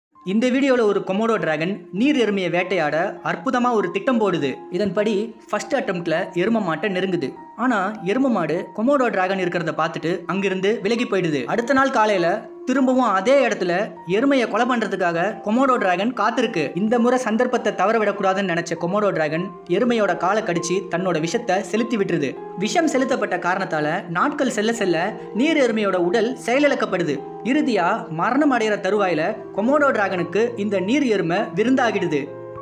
Komodo Dragon Eats Buffalo 😱
keep watching @Mojoreels

A big part of a Komodo dragon's diet is carrion, or decaying flesh from dead animals. But they are fully capable of hunting live prey, and there are recorded instances of fully-grown Komodo dragons taking down animals as large as horses and water buffaloes.